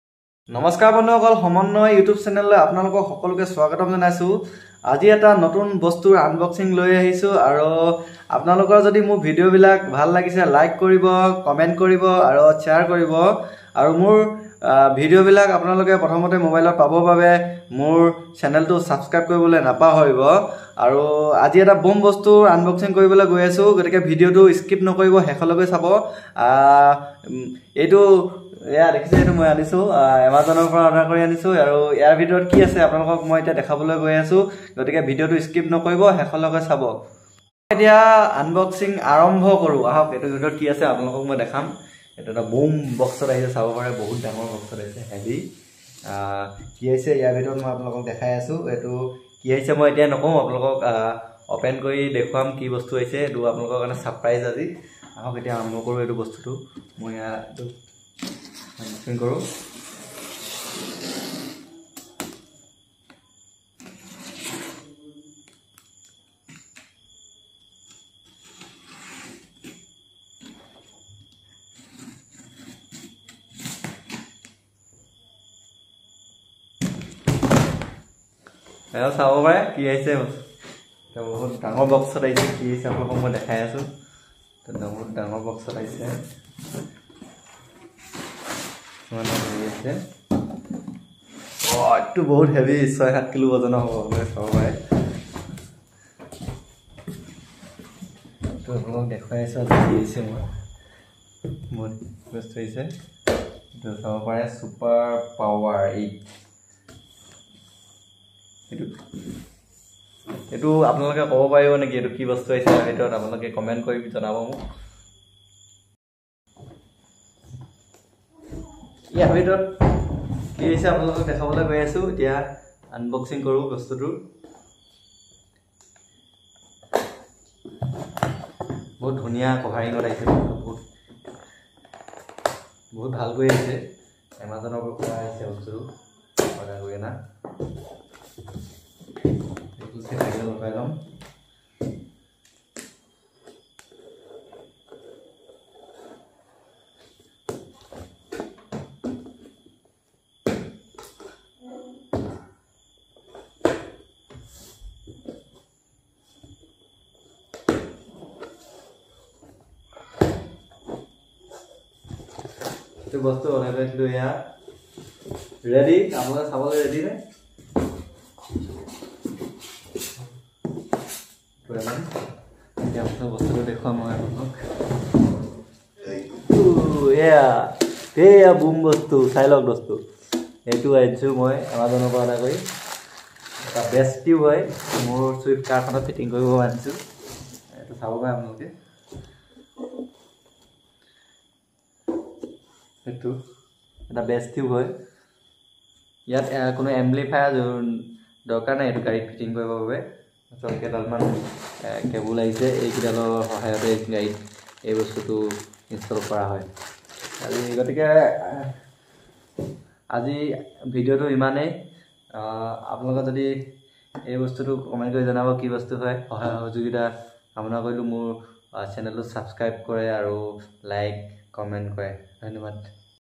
UNBOXING a Bass tube /KIANO Supreme Power 8 4500 watt
I'm buying this product from Amazon